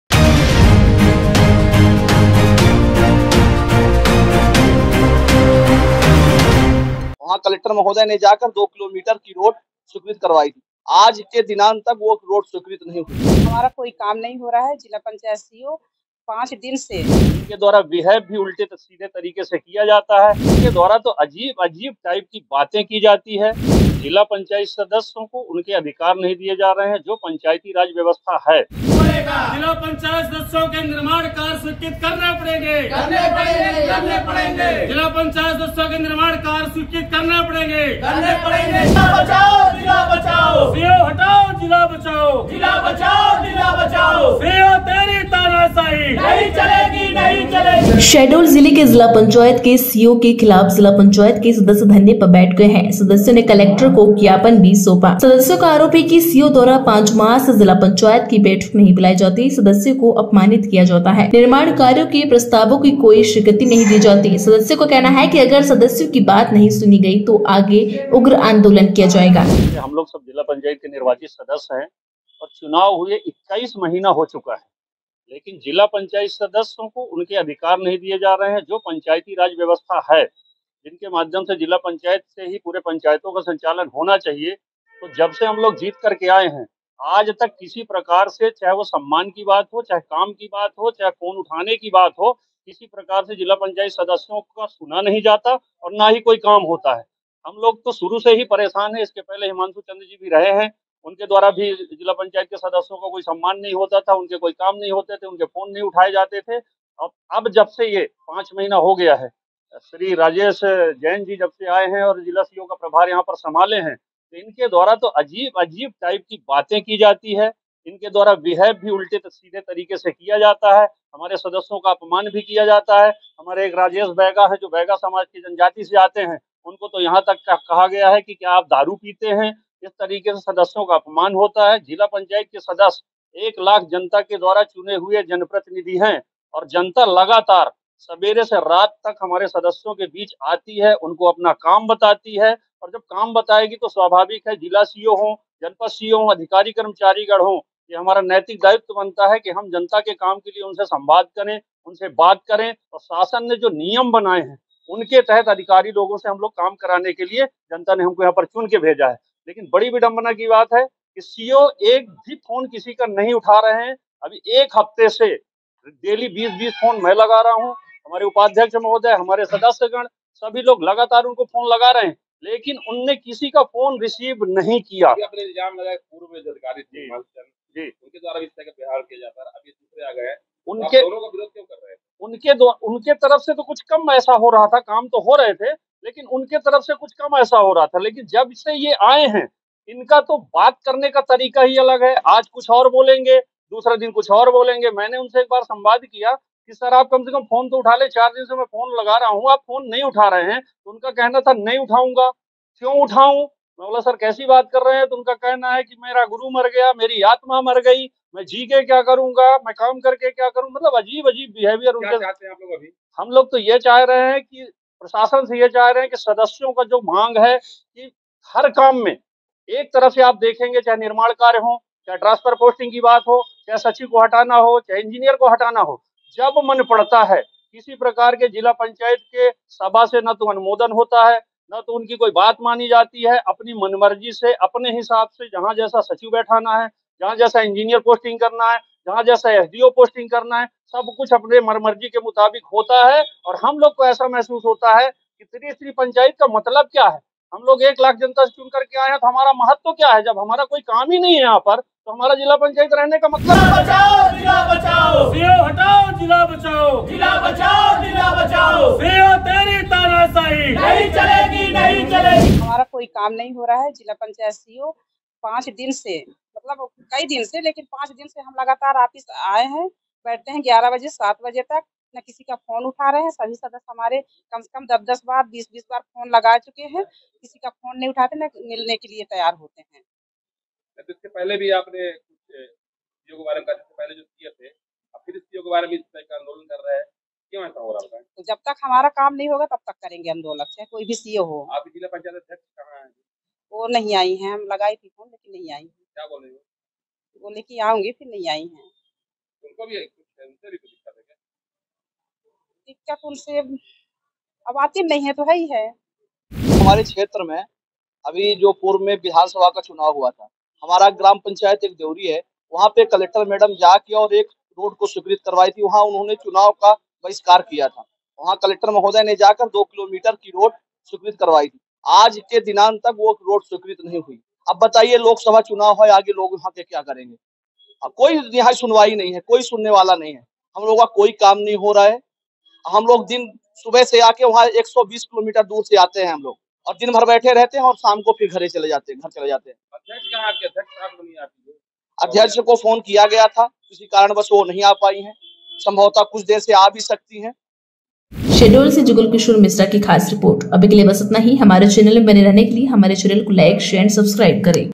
CEO पर सदस्यों को अपमानित करने, बैठक न बुलाने, फोन न उठाने का लगाया का आरोप The Khabardar News
सदस्यों द्वारा CEO पर अपमान करने, बैठक न बुलाने, और फोन न उठाने का आरोप लगाया गया है। इस विवाद में और जानकारी के लिए पढ़ें।